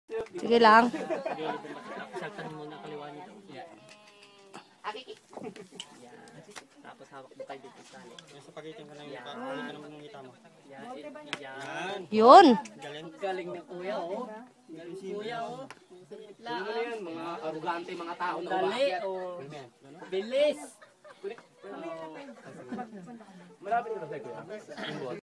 Pag-akyat sa Monolith gamit ang Lubid Part 2
It's my my turn.

Kailangan mong gumamit ng lubid para makaakyat ka sa tuktok ng Monolith. Congratulations sakin!